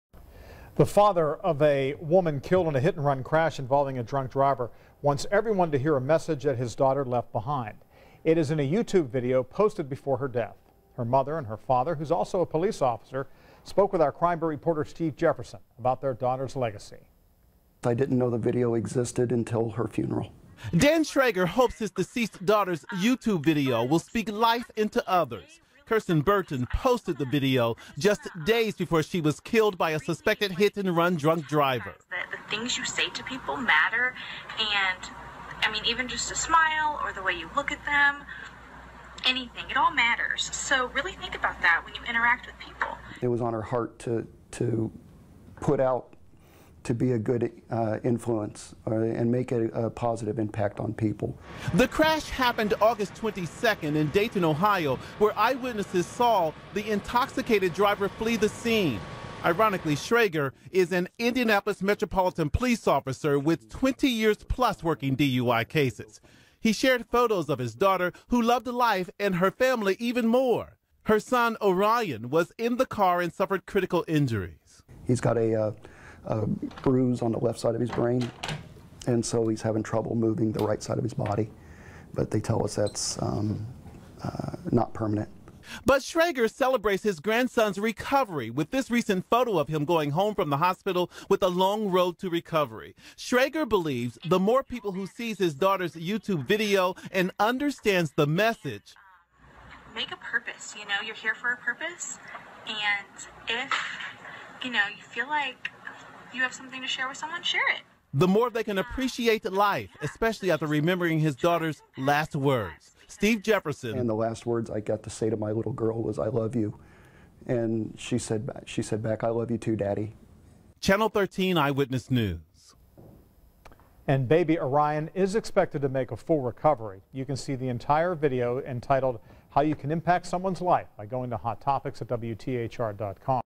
Daughter's message
The father of a woman killed in a hit and run crash involving a drunk driver wants everyone to hear the message his daughter left behind.